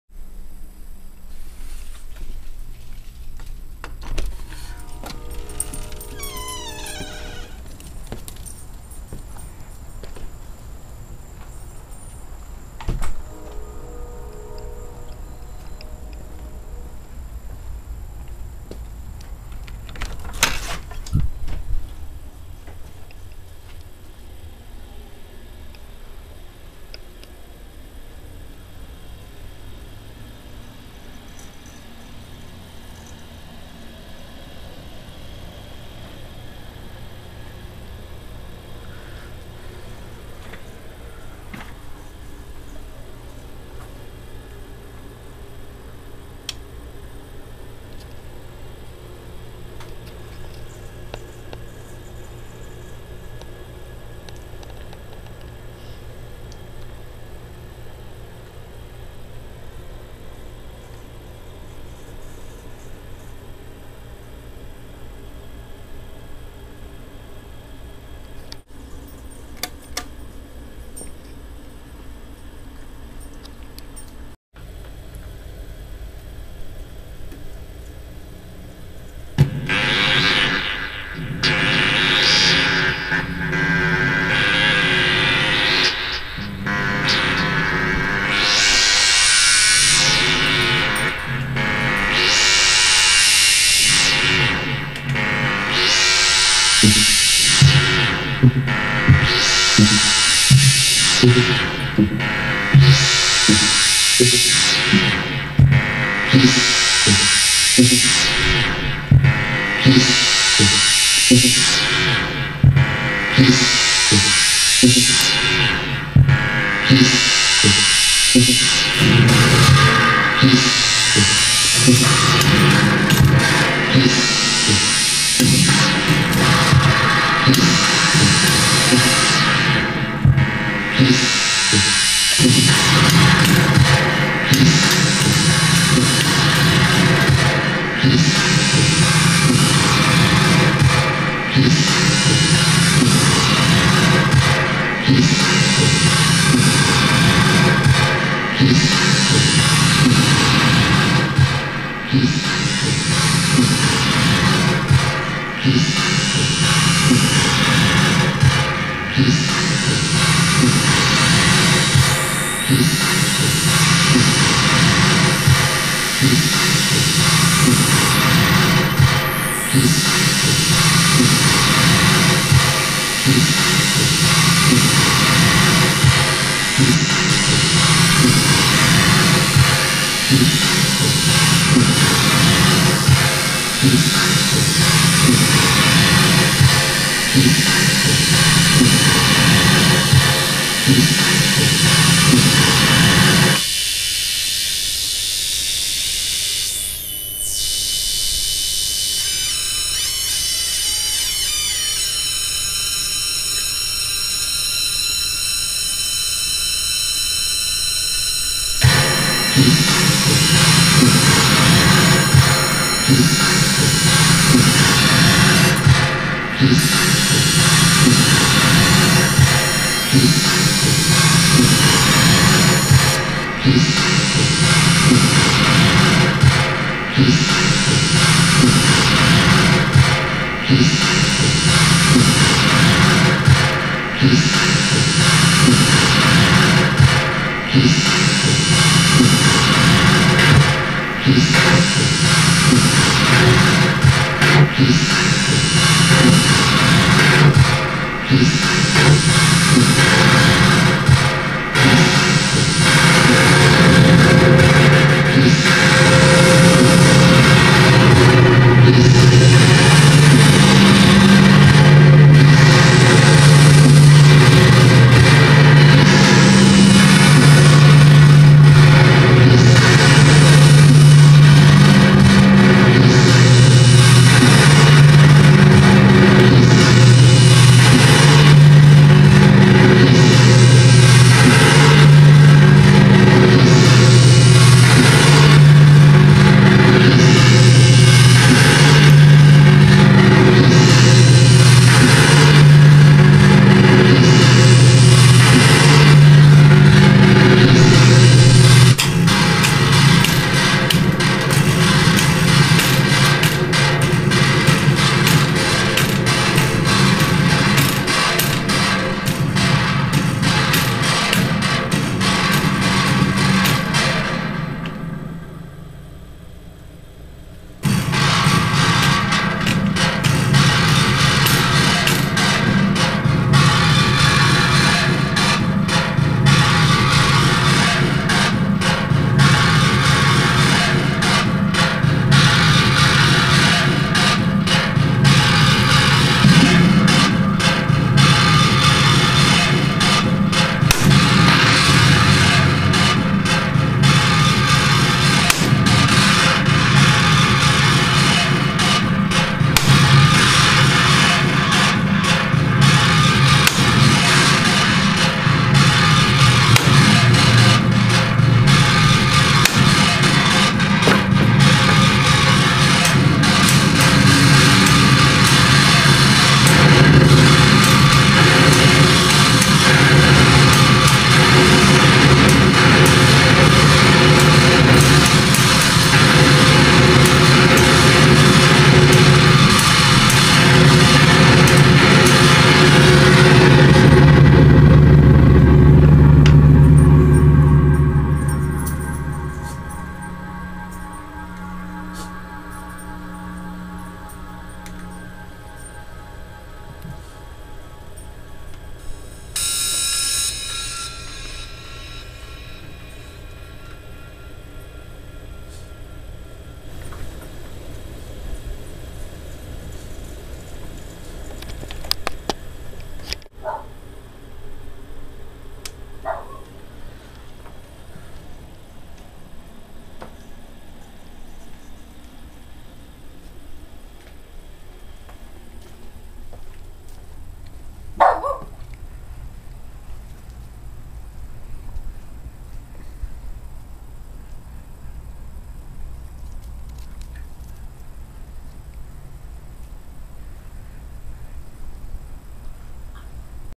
This is what I do...
A short film/music video of an original improv performance in my garage. Shot, directed and edited by me.
Instruments used: Korg MS2000B, Monotron, KP-3, an instrument I built called TheThing and various pedals and effects, mainly the Loopstation.
Also featured at the end, are my three dogs, The Dude, Maude and Judah.
That's Duder that walks up to me at the end.